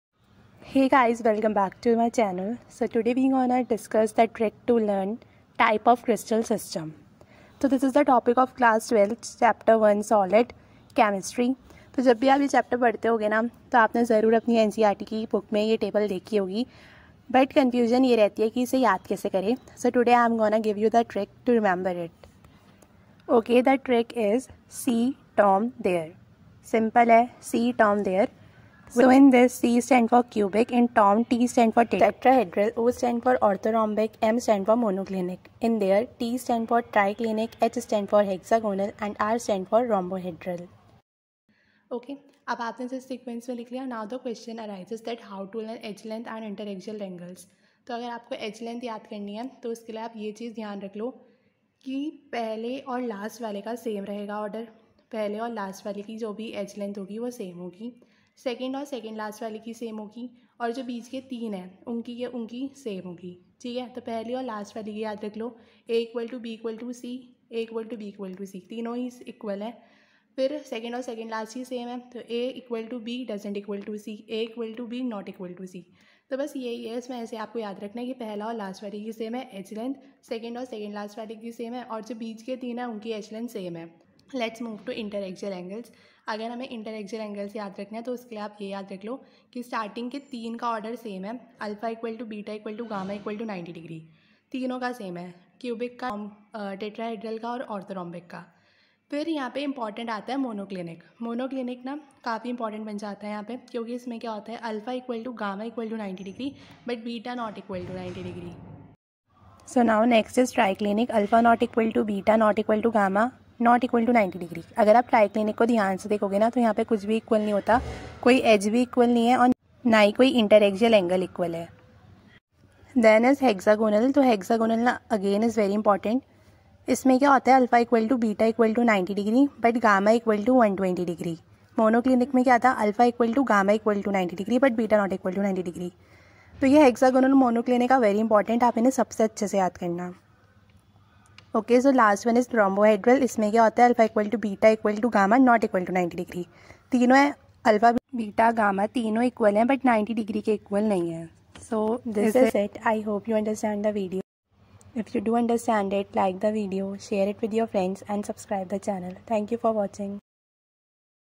TRICK --type of crystal system ||ch solids|| class 12
this particular video is about the trick to remember type of crystal system.